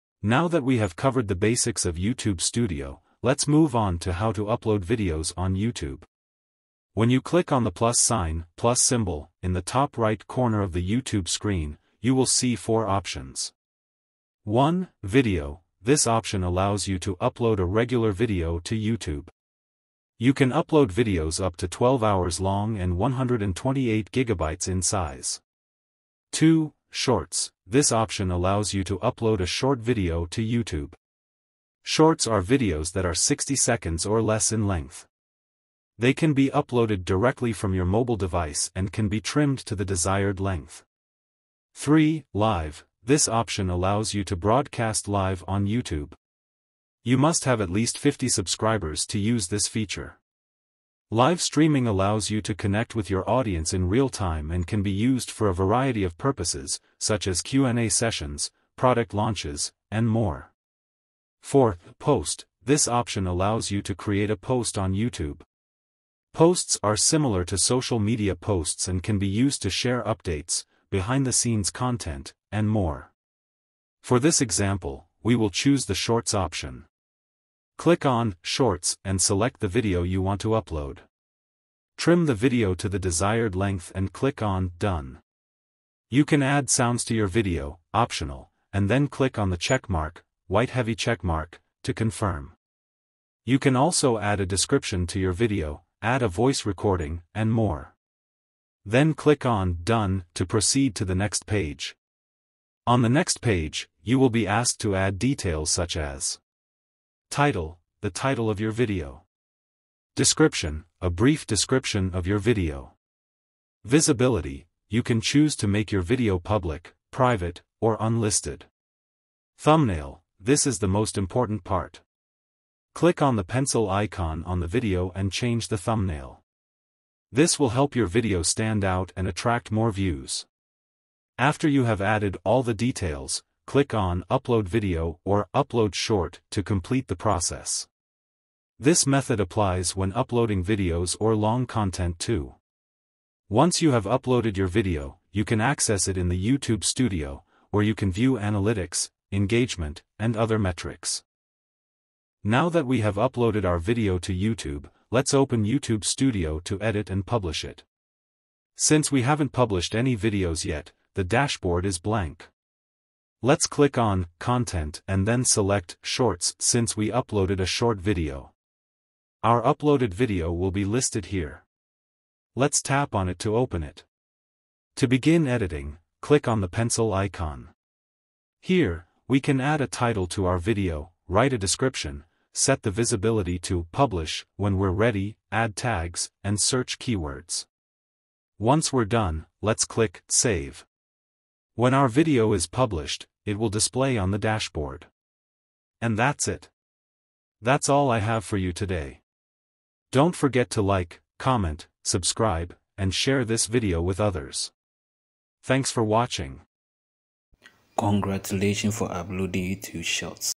How to Upload Videos to YouTube: A Step-by-Step Guide
Learn how to upload videos to YouTube with this easy-to-follow guide. This video will walk you through the process of uploading videos to YouTube, including how to create a YouTube account, how to upload videos, and how to optimize your videos for search. Whether you're a beginner or an experienced YouTuber, this video will provide you with the knowledge you need to upload videos like a pro.